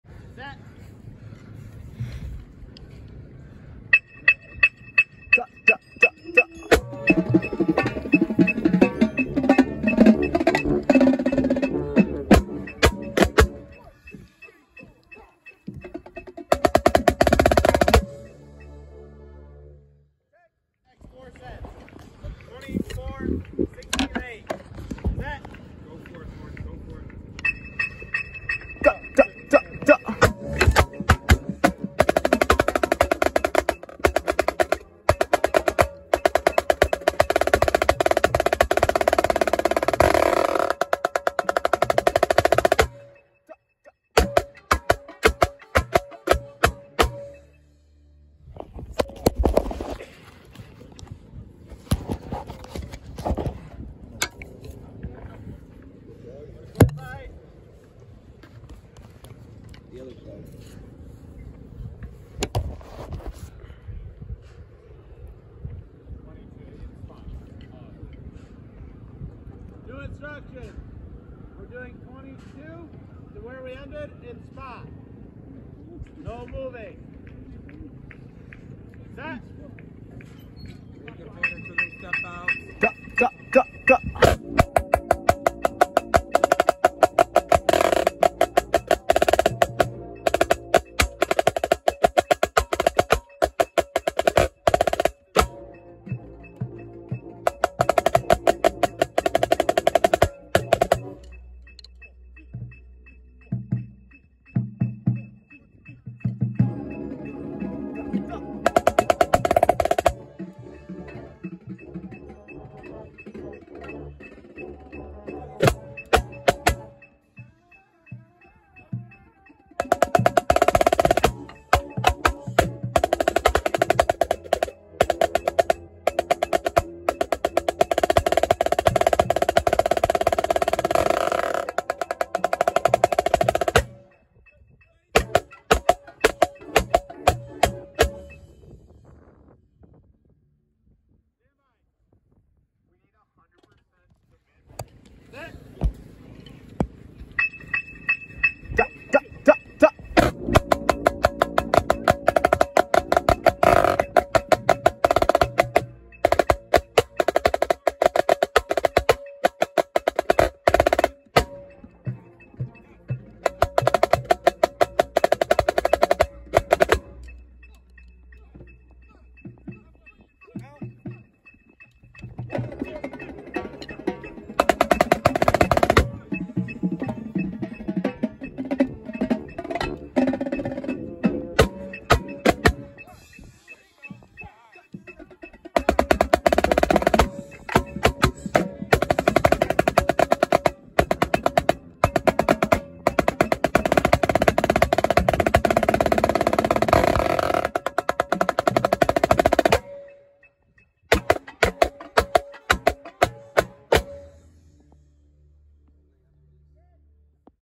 Spring Camp 4/21/24: moving and playing Scene 2
CT hurricanes putting together our scene 2 segment moving and playing on the field